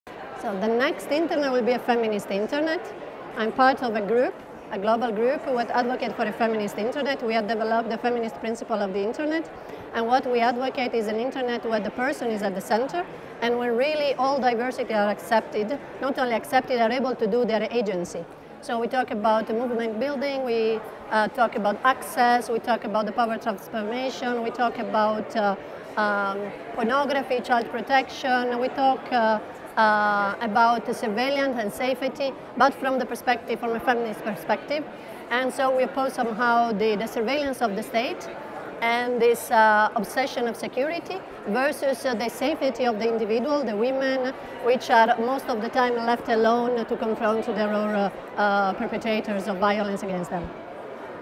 Global IGF 2016: Valentina Hvale Pellizzer on Next for the Future of the Internet
Valentina Hvale Pellizzer, President, One World Platform, shares her answer to the question, "What do you think the next big thing in the Internet's evolution will be?" Recorded at the Global Internet Governance Forum 2016 in Guadalajara, Mexico, by the Imagining the Internet Center, an initiative of Elon University, North Carolina, USA.